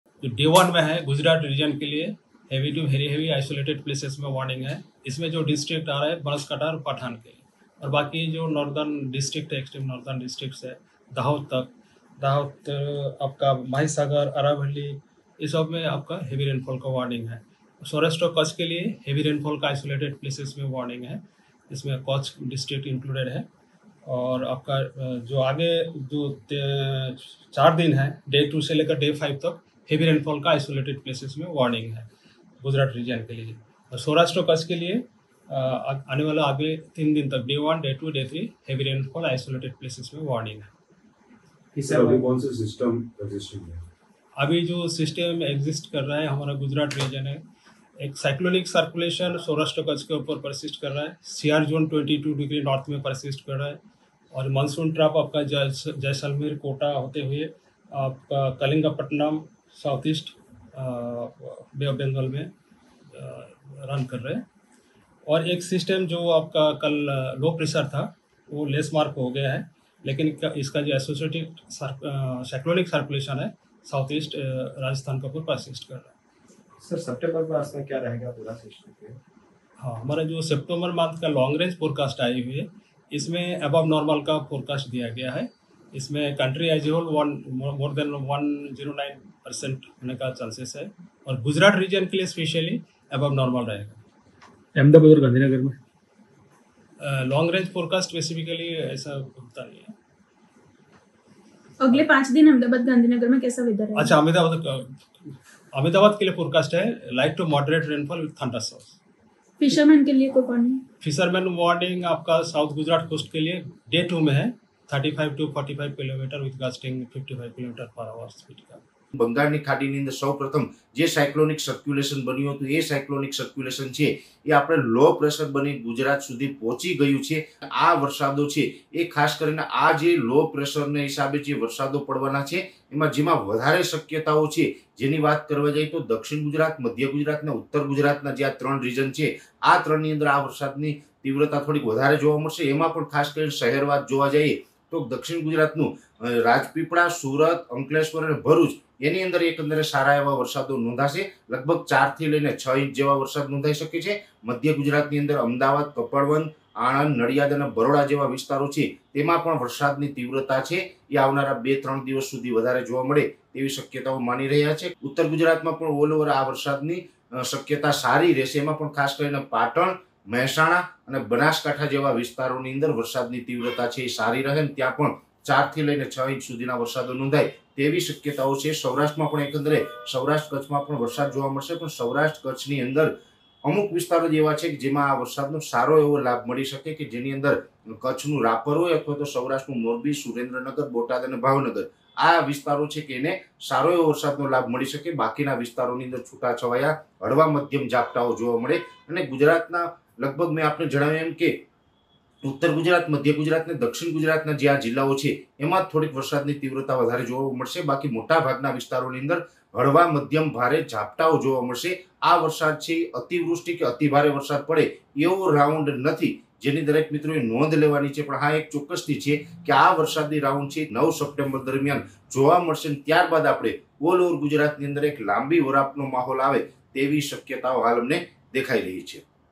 Gujarat Rain | ભારેથી અતિભારે વરસાદની આગાહી! જાણો ક્યાં | Weather | Heavy Rain | News 18 | N18V
Weather forecast for rain. Clouds can cause thunder in North Gujarat.

વરસાદને લઈ હવામાન વિભાગની આગાહી. ઉત્તર ગુજરાતમાં મેઘો બોલાવી શકે ધડબડાટી.n18oc_weather